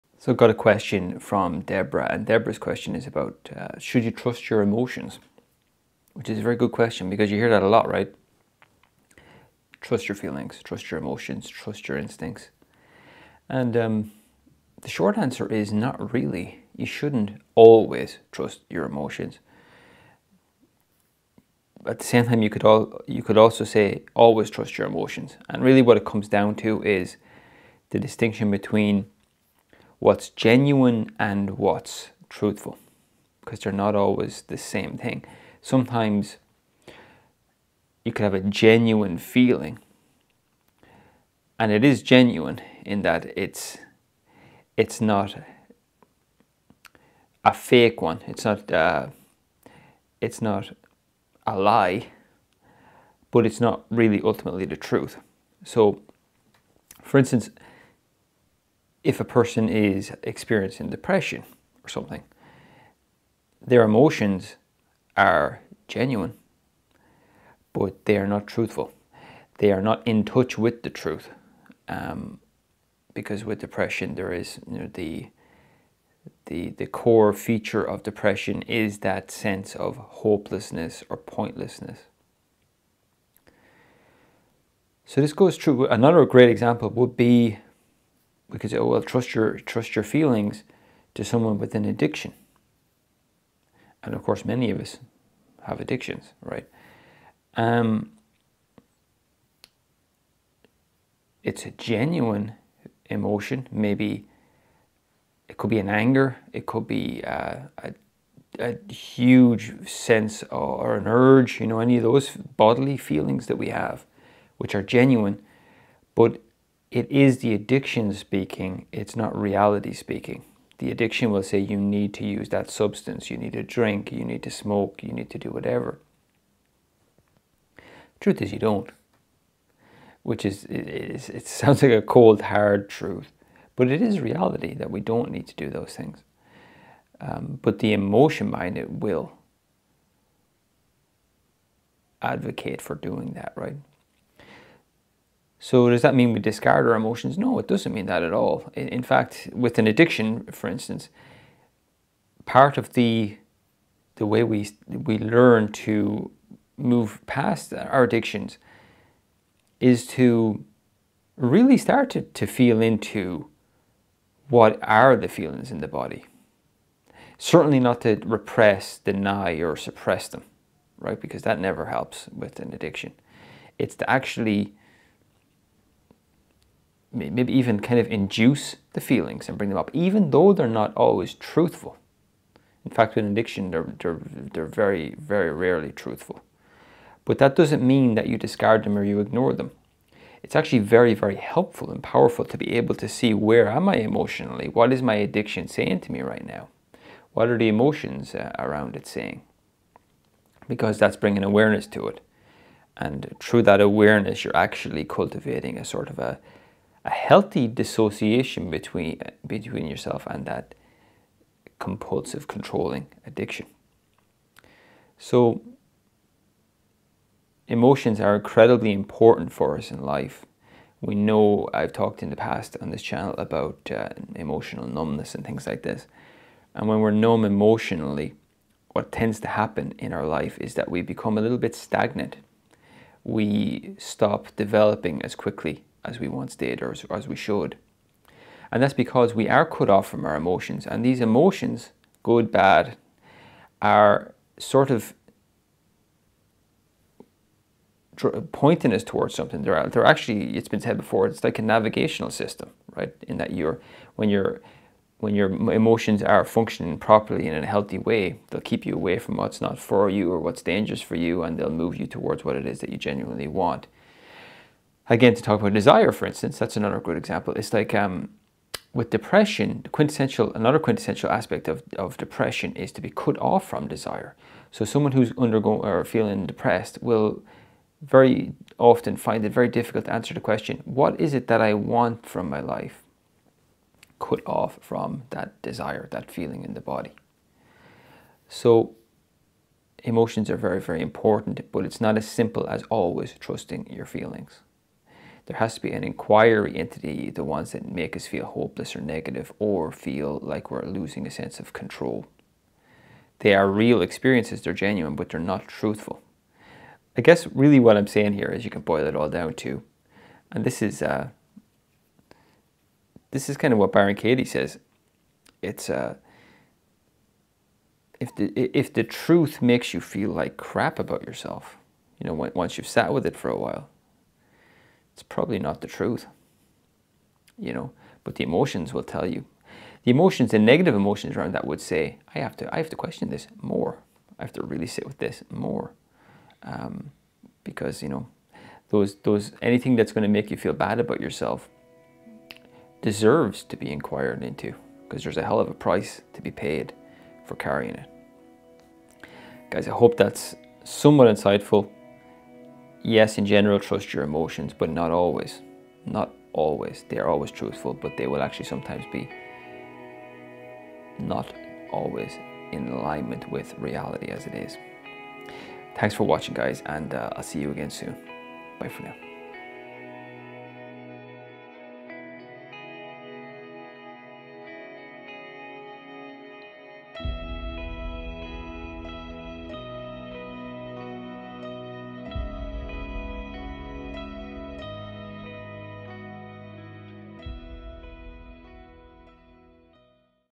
Should You Always Trust Your Emotions?
We've all heard that 'you should always trust your emotions.' In this video, I try to put a finer point on this piece of advice.